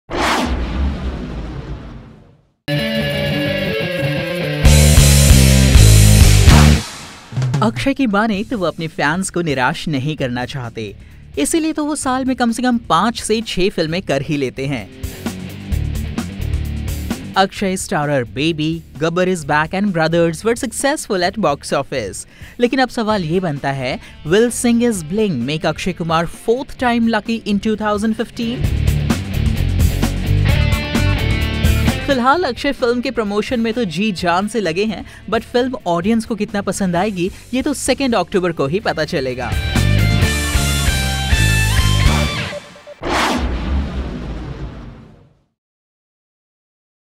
Will Akshay be 4th time lucky?
After delivering back to back hits like Baby, Brothers and Gabbar is Back, will the Khiladi of Bollywood be lucky with his forth film too?